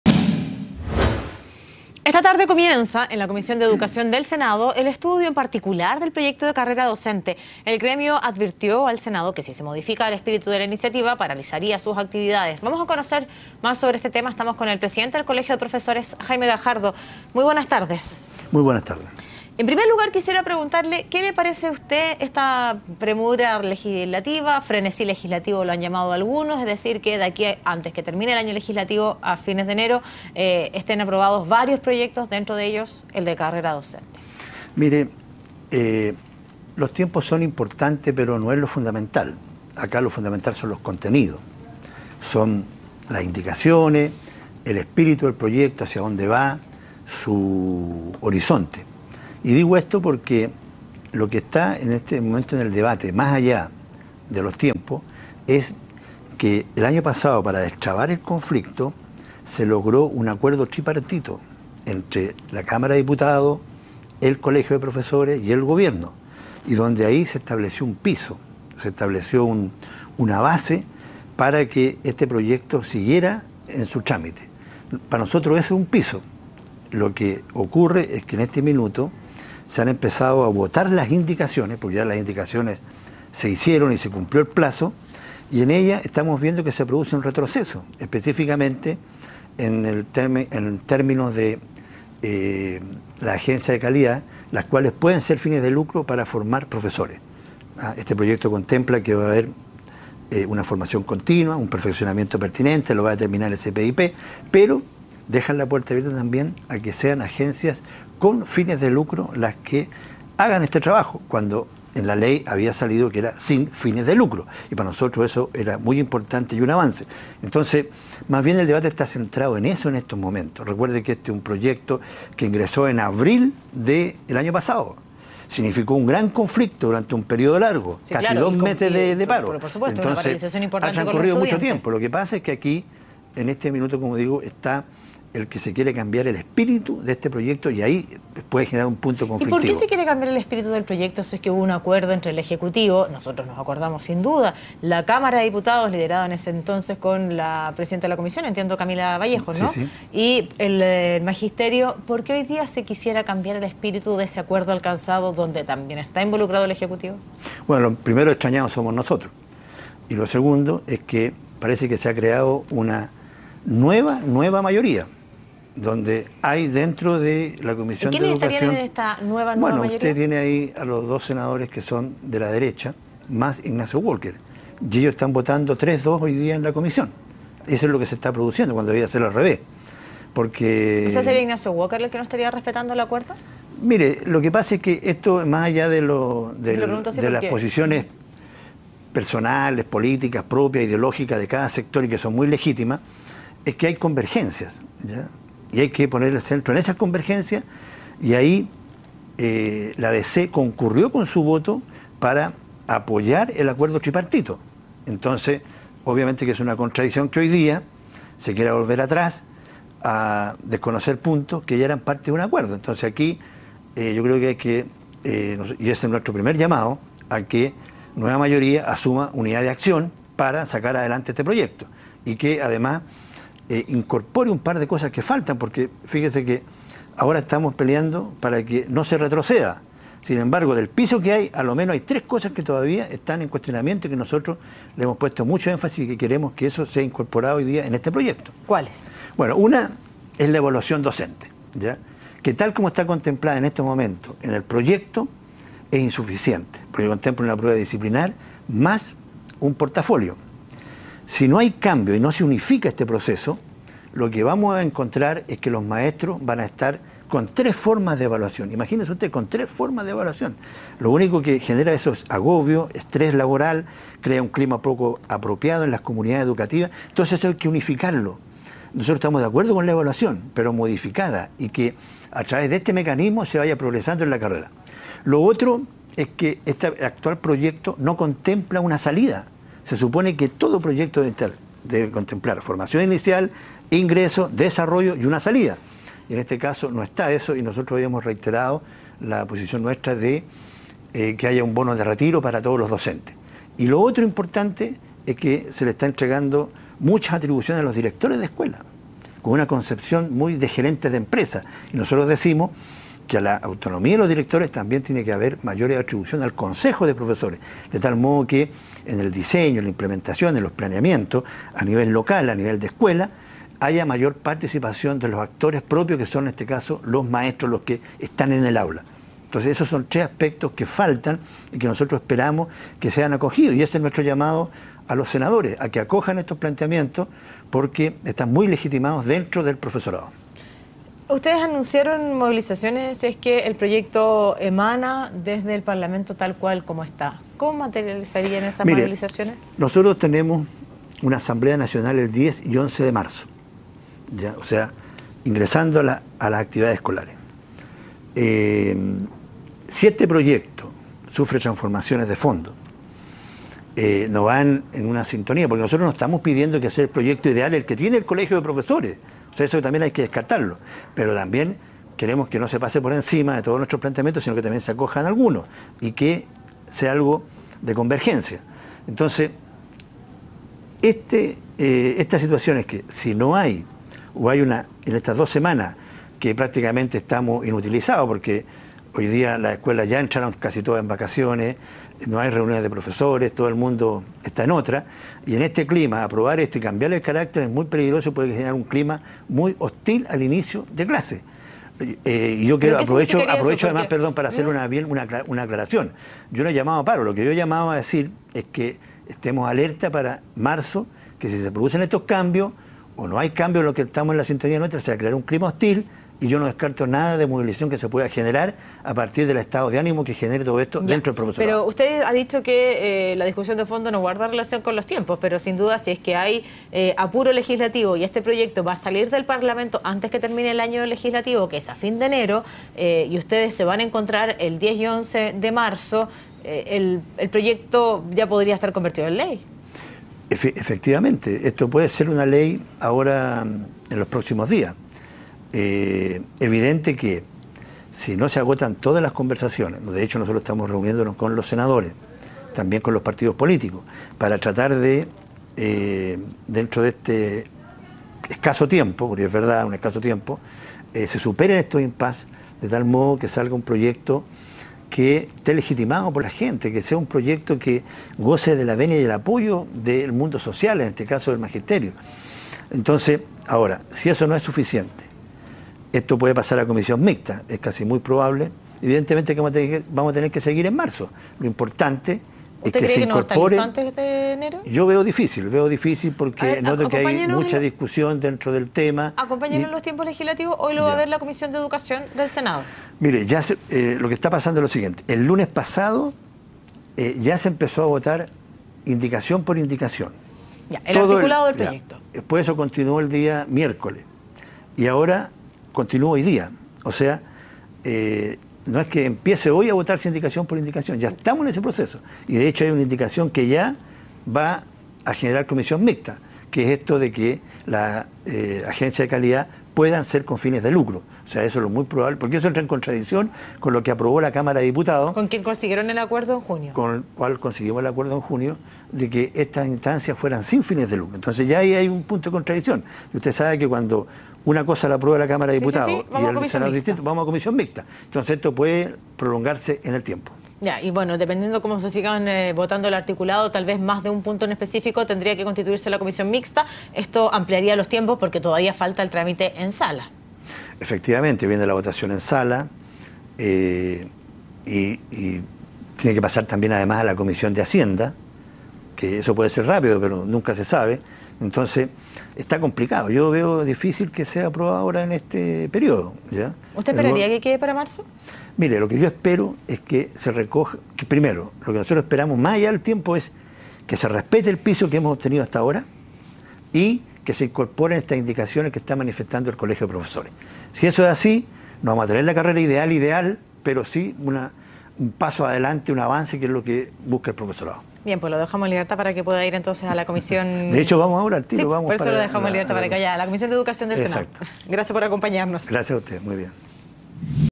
La postura de los profesores ante el proyecto de carrera docente
En la iniciativa trabajaron docentes, el ministerio de Educación y la Comisión de Educación de la Cámara baja.